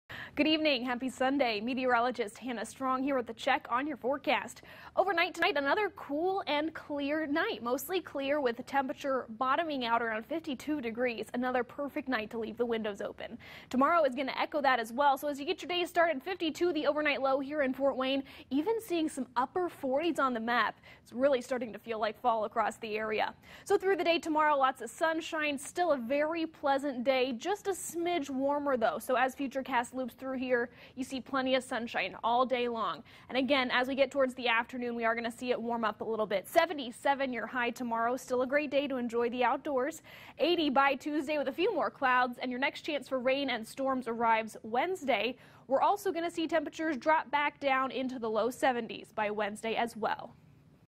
WANE Sunday Night Forecast
Mostly clear and cool tonight.